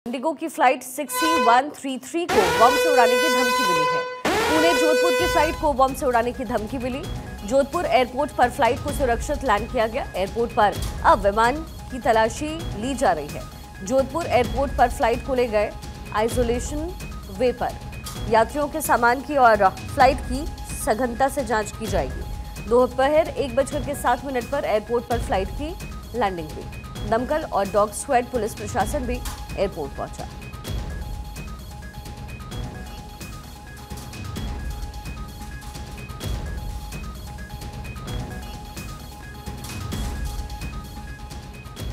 Indigo की फ्लाइट 6E133 को बम से उड़ाने की धमकी | Pune-Jodhpur Route | Bomb Threats | Latest News
Breaking News: इंडिगो की फ्लाइट 6E133 को बम से उड़ाने की धमकी

Breaking News: Bomb threat to IndiGo flight 6E133

Anchor Name : Shikha Thakur | Show Name : News | Time : 03:00 PM | Date : 20/10/2024

Published By: Sujal Singh